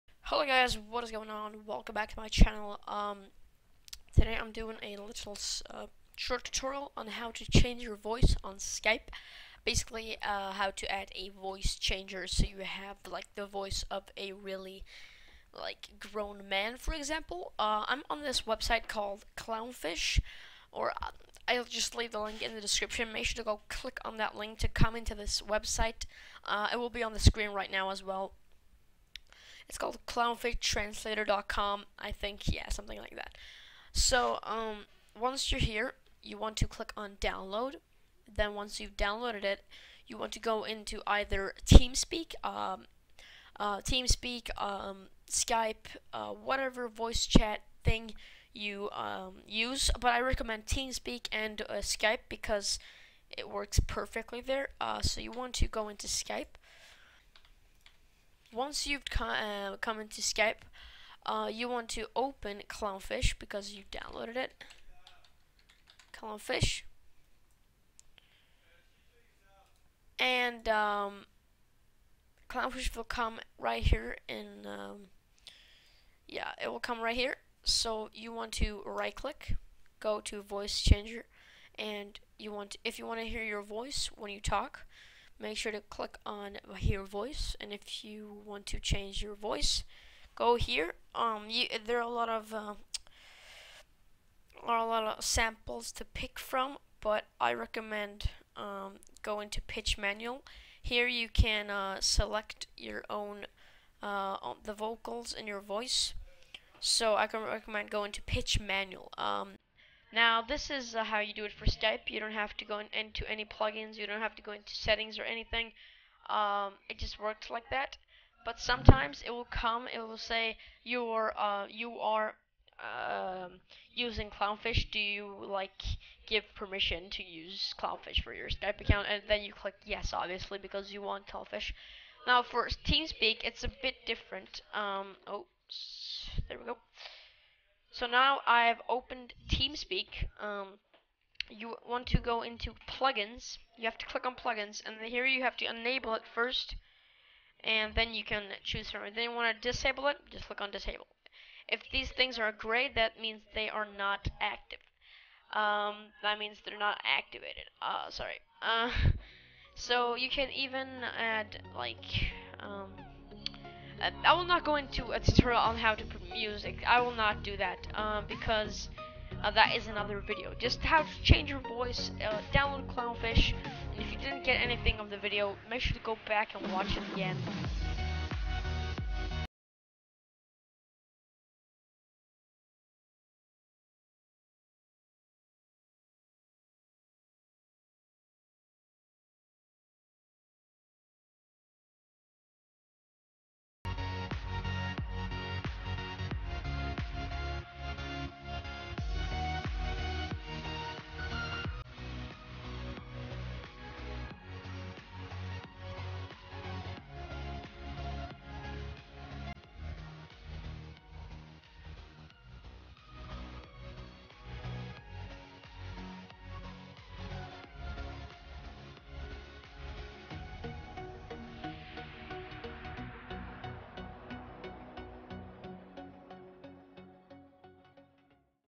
How To Change Your Voice On Skype Or Teamspeak (Even other voice chat softwares!) 2017!
Please click on the thumbs up button, just so I know if you guys want more tutorials!